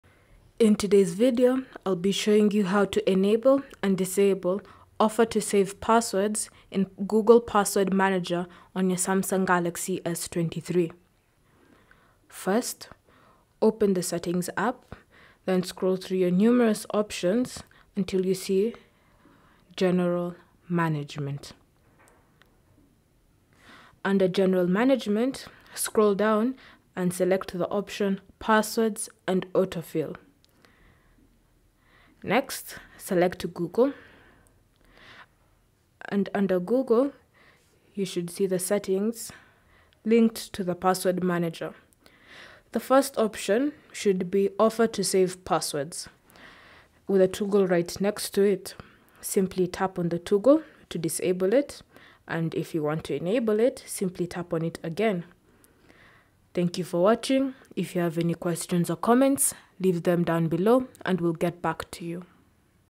How To Enable / Disable 'Offer to Save Passwords' In Google Password Manager On Samsung Galaxy S23s
Learn How To Enable & Disable 'Offer to Save Passwords' In Google Password Manager On Samsung Galaxy S23, S23+, S23 Ultra. In this video tutorial, we'll walk you through the steps to enable or disable the 'Offer to Save Passwords' feature in Google Password Manager on your Samsung Galaxy S23.

Whether you want to streamline your password management or prefer to have more control over it, we've got you covered.

How To Enable 'Offer to Save Passwords' In Google Password Manager On Samsung Galaxy S23

1. Open the Settings app

2. Next, tap on General Management

3. Under General Management, select Passwords and Autofill

4. Select Google

5. You should see the option Offer to Save Passwords with a toggle next to it.

6. Tap on the toggle to turn it on. It should turn blue to show it is enabled.

How To Disable 'Offer to Save Passwords' In Google Password Manager On Samsung Galaxy S23

1. Open the Settings app

2. Next, tap on General Management

3. Under General Management, select Passwords and Autofill

4. Select Google

5. You should see the option Offer to Save Passwords with a toggle next to it.

6. Tap on the toggle to turn it on. It should turn blue to show it is disabled.

Cell Phone Tripod Adapter